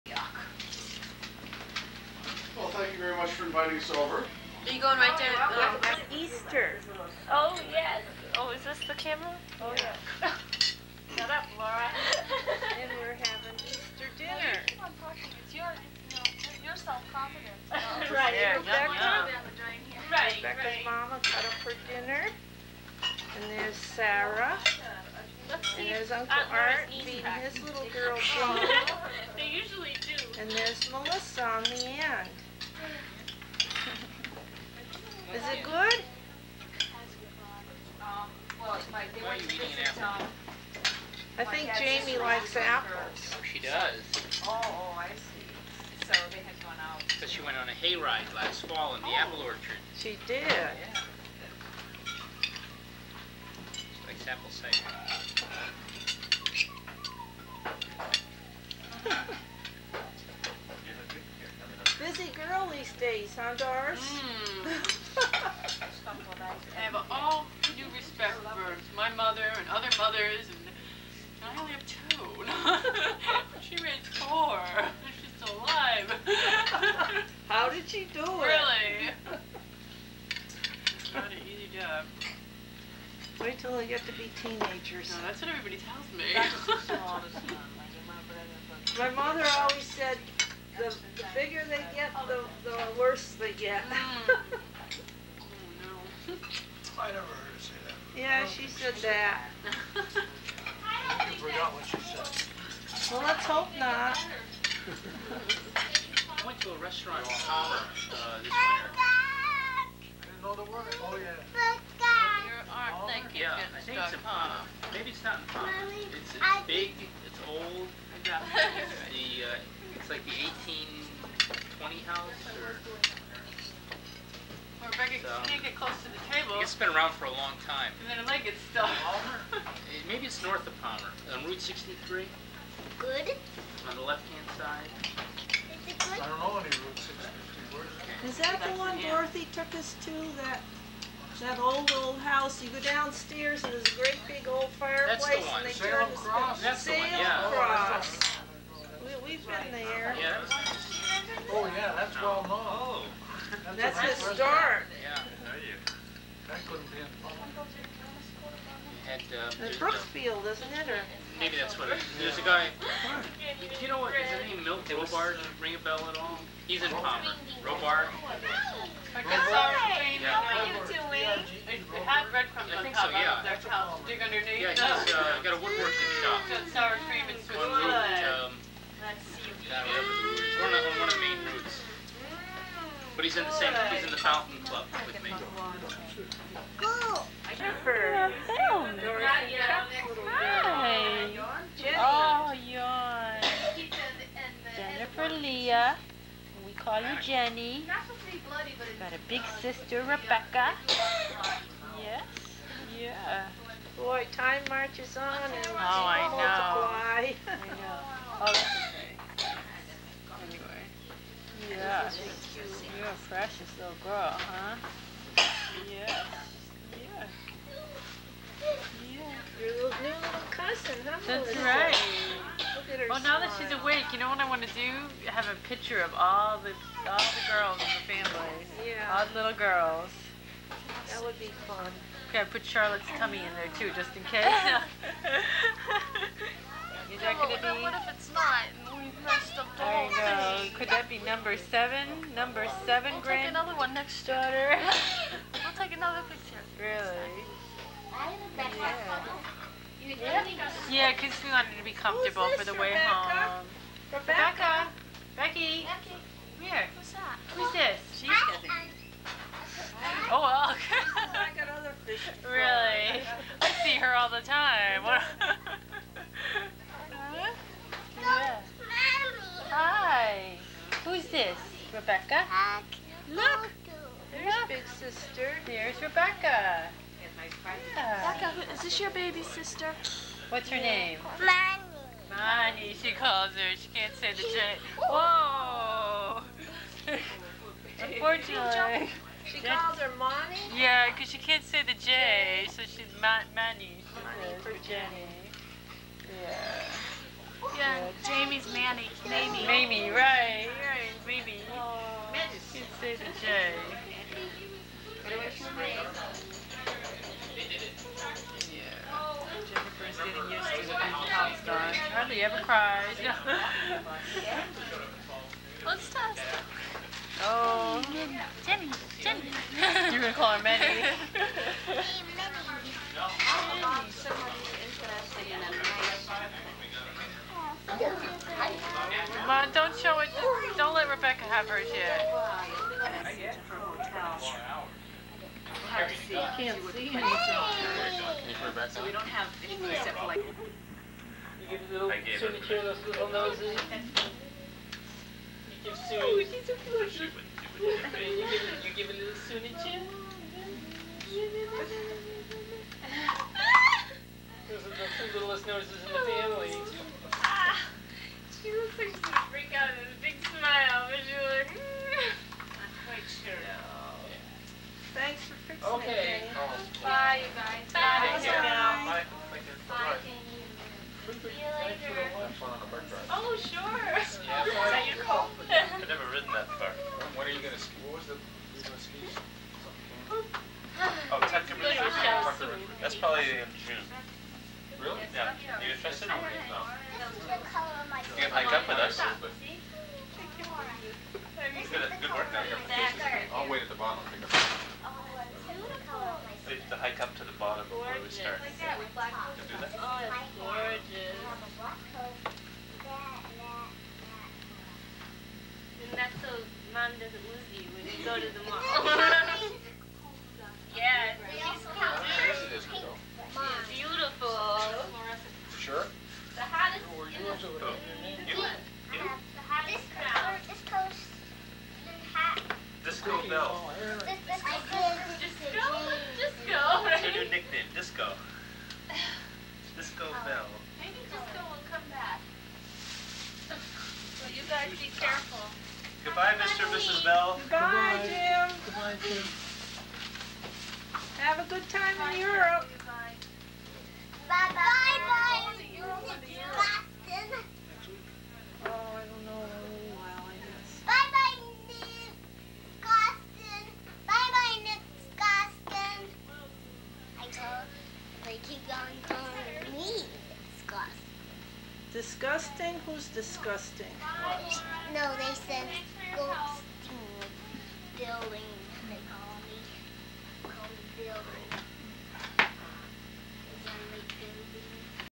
1990EasterAtKenChar
From Mom and Dad's (Ruth and Charlie Bell) video collection. Easter at Ken and Charlotte's home in Townsend, MA, April 1990